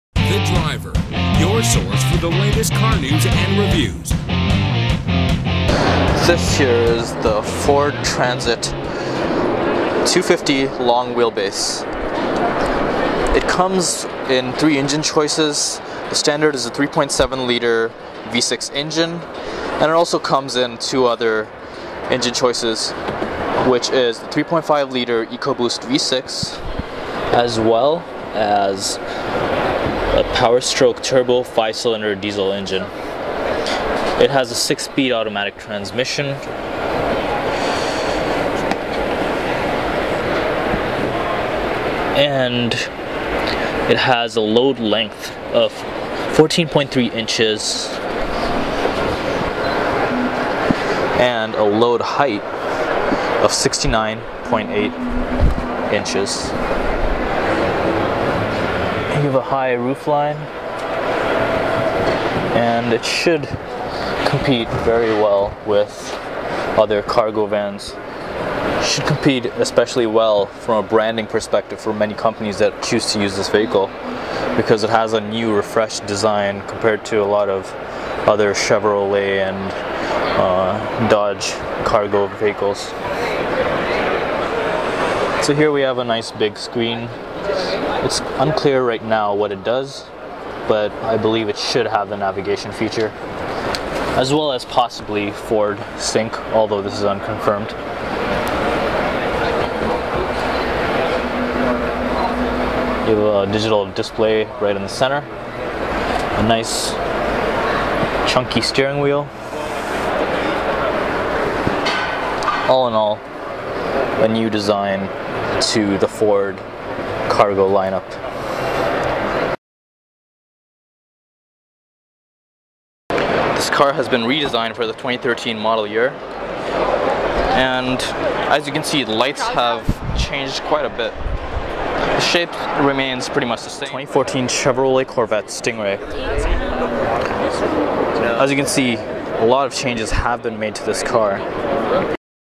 2014 Ford Transit Van at the Detroit Auto Show - The Driver
A first look at the Ford Transit van at the 2013 North American International Auto Show in Detroit. This van is a cargo van that is making its North American debut after being sold in Europe for a while. Built with three engine choices, it competes directly with the Chevrolet Express and the Dodge Sprinter.